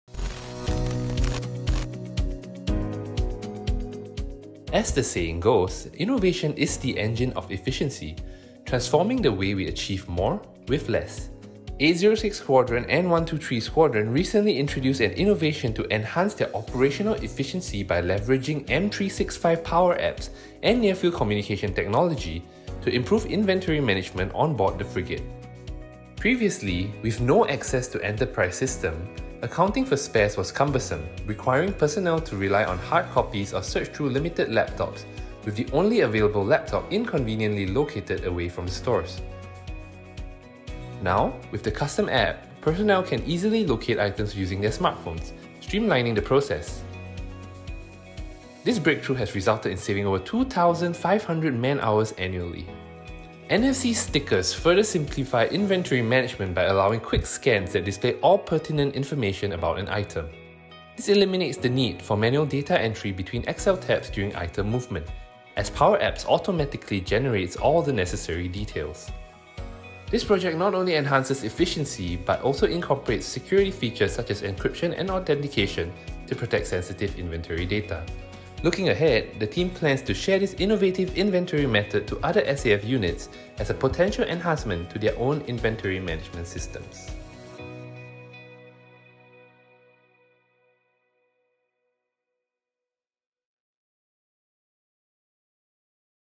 The “N.I.C.E” App (An RSAF Innovation)
Introducing the “N.I.C.E” App – an innovative

NFC (Near Field Communication)
Inventory
Collaborative
Effort

Video by PTE Lai Mun Hin